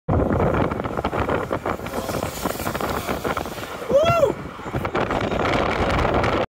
Hot Brakes
Truck overheated the brakes and lit the truck on fire @topsaw